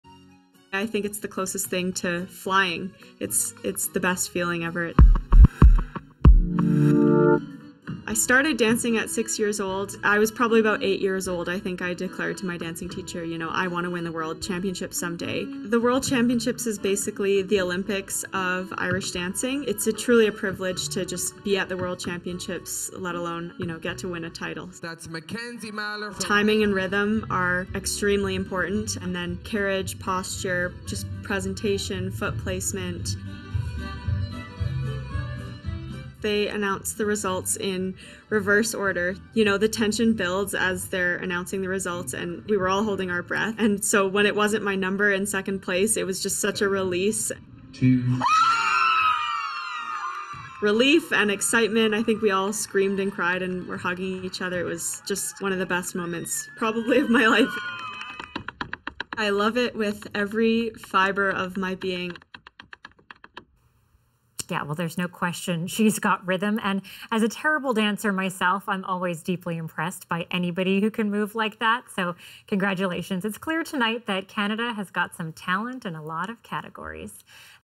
TheMoment Calgary woman wins Irish Dancing World Championship
Mackenzie Mahler finished in first place in the senior ladies category at the World Irish Dancing Championships held earlier this month in Montreal. It's a childhood dream come true for the Calgary woman, who has been dancing since she was six years old.